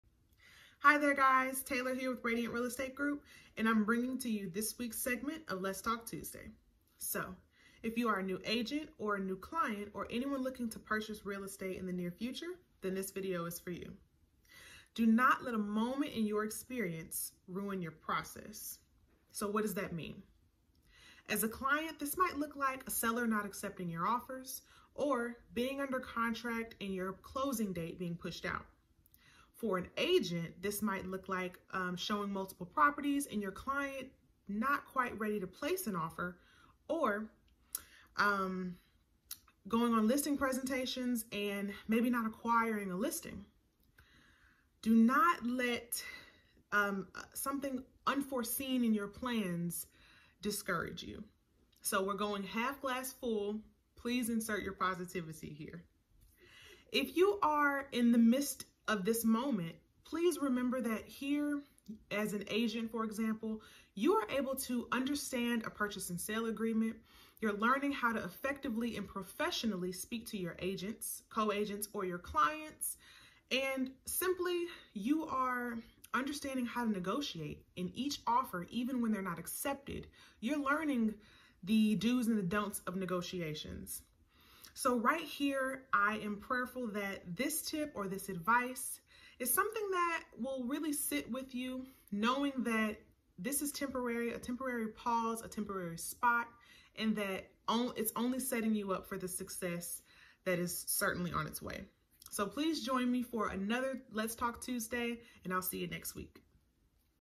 Let's Talk Tuesday!
Hi Rays!

Watch Tay’s update video! 🤗✨

Don’t let those specific moments in your Real Estate journey ruin your entire process! That “moment” is strictly temporary and grounds for learning and expanding upon your already existing knowledge.

It WILL you grow!

Trust your selected Agent and the process✨🔥

Do you want to buy or sell? Give us a call today!”

Century 21 Premier Real Estate